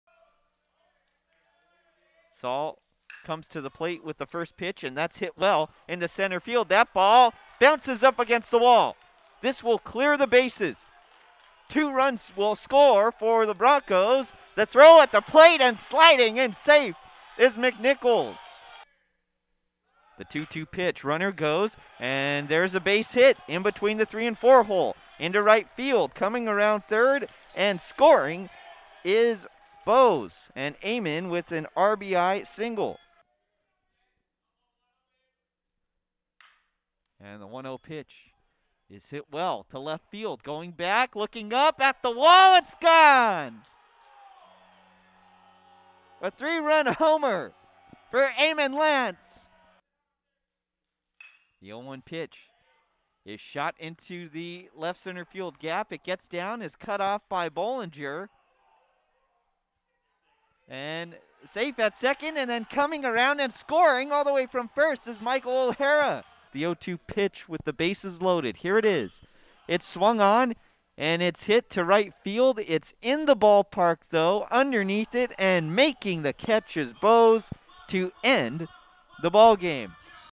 HIGHLIGHTS: Santa Clara Baseball Defeats Sacramento State | March 7, 2021
The Broncos scored 10 runs in 12 hits in the 10-6 victory over the Hornets at Schott Stadium.